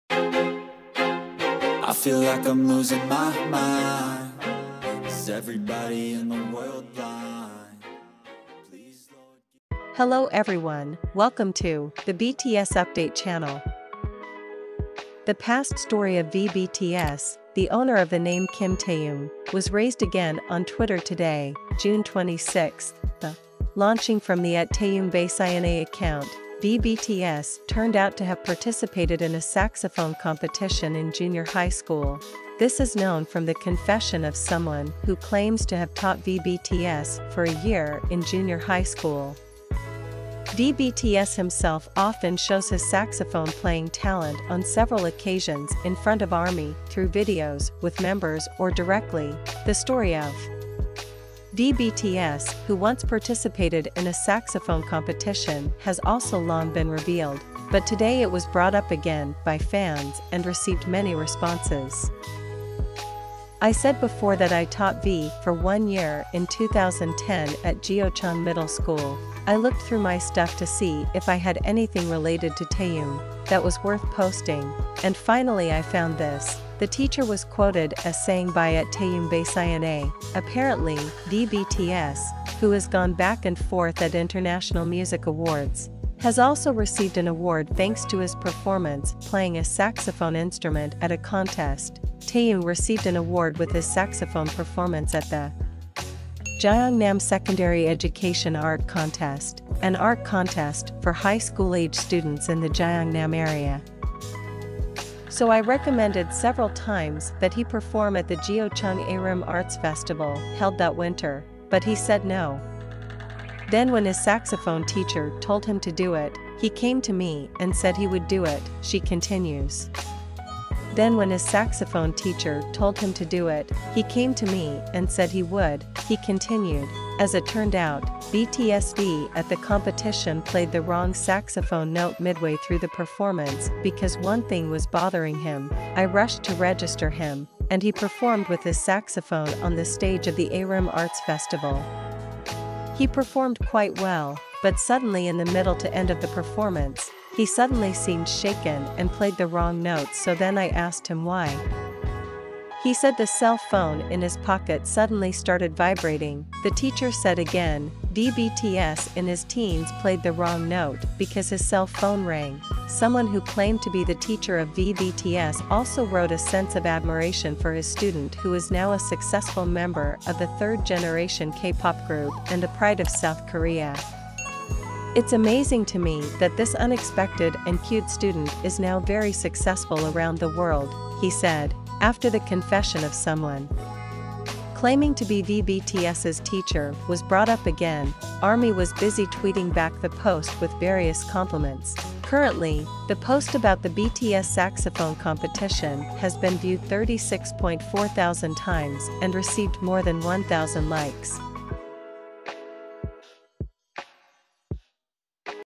Today's bts news! Netizens are shocked that someone V BTS' teacher's confession.
The past story of V BTS, the owner of the name Kim Taehyung, was raised again on Twitter today (26/06).
Launching from the @TaehyungBaseINA account, V BTS turned out to have participated in a saxophone competition in junior high school.
This is known from the confession of someone who claims to have taught V BTS for a year in junior high school..